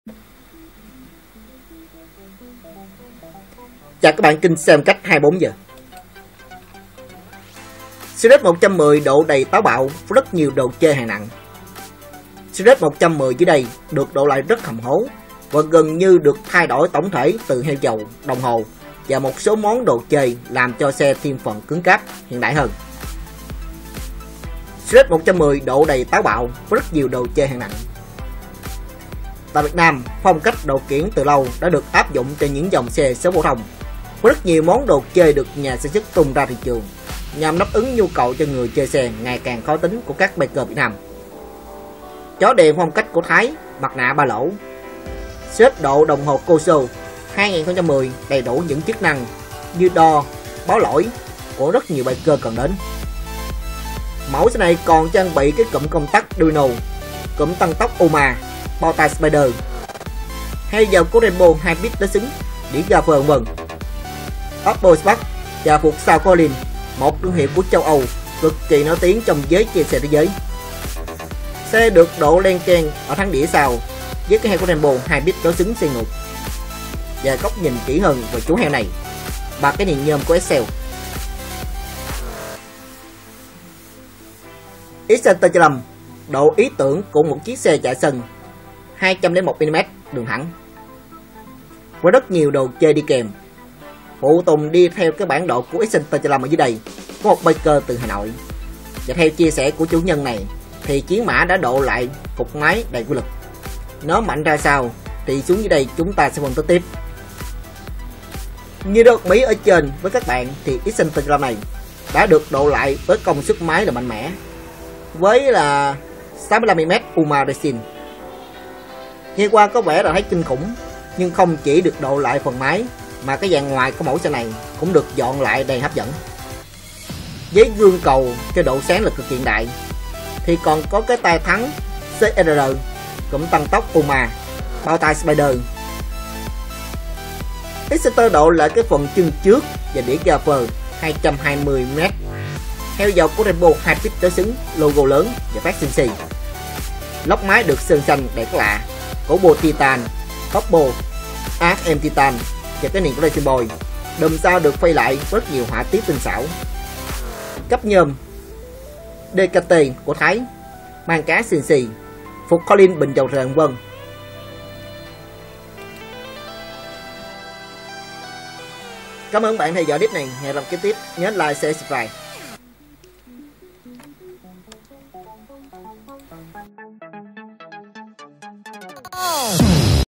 Xe & Phong Cách 24h - Sirius 110 độ đầy bá đạo với nhiều đồ chơi hạng nặng.
Sirius 110 dưới đây được độ lại hầm hố và gần như đã được thay đổi tổng thể từ heo dầu, đồng hồ và một số đồ chơi khác cho xe thêm cứng cáp và hiện đại hơn.